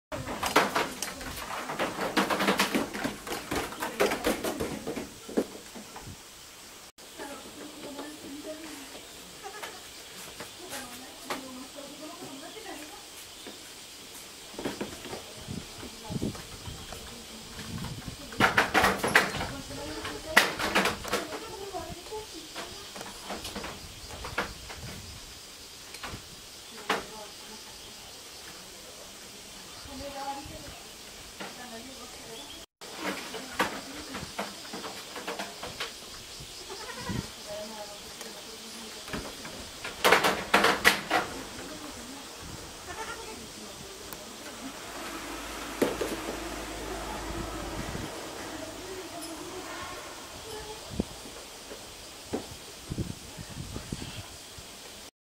丸太橋を駆ける子ヤギ達
２号丸太橋

高い所が大好きなので

競争して丸太橋を渡る

子ヤギのkikiキキ ＆ Latteラテ 姉妹

初めて渡った時に妹Latteラテが躊躇していたのは何だったのか？

Kid goat crossing Log Bridge

Goats running on the log bridge

Kid goats running on the log bridge